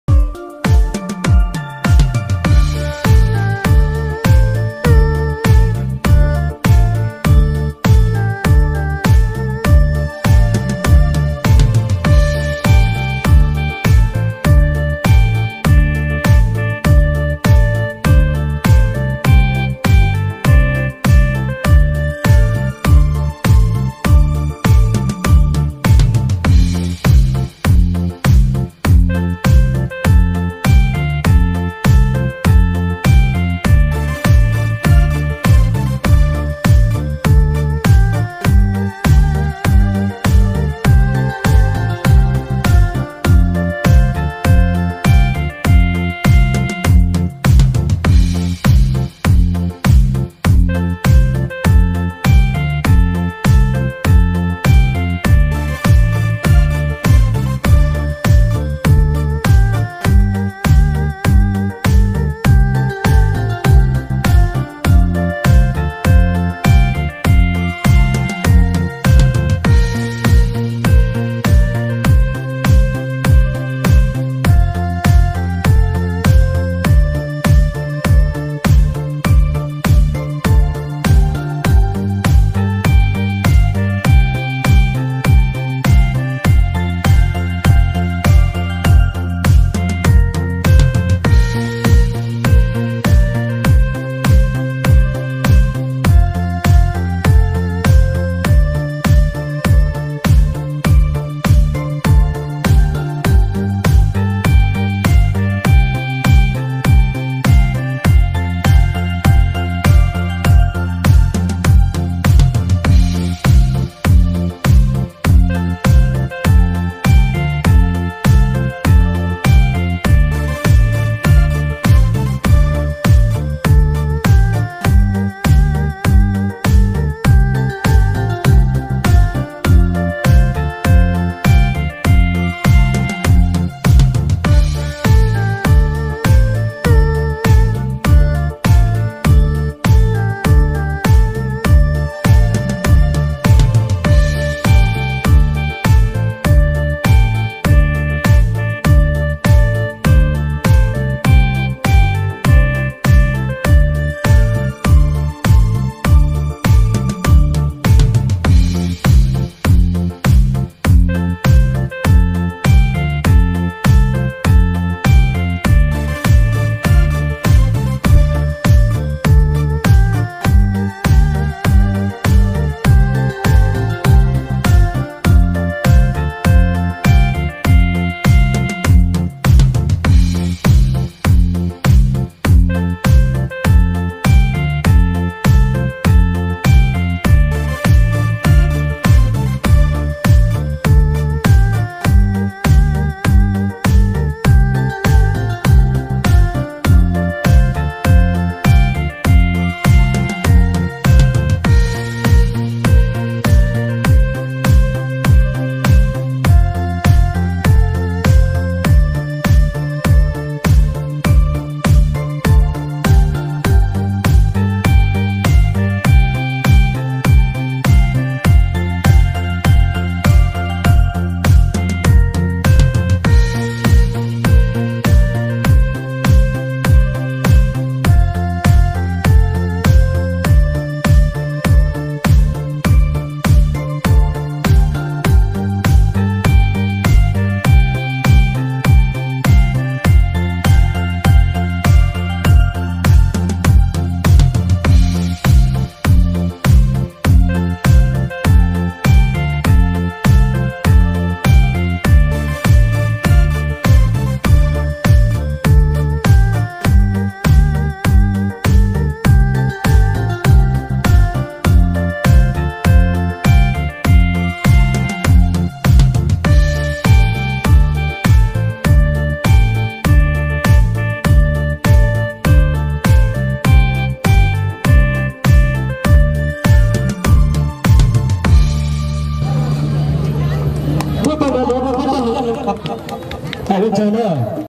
màn nhảy Shuffle nóng hết cả người các cô gái H'mông xinh đẹp nhảy - than Uyên 2/9 (SK T)
camera edit nuj swim
nhảy Nkauj Hmoob than Uyên

chúc mọi người xem video vui vẻ nha cảm ơn mọi người đã ủng nhóm SK T mình nha 🥰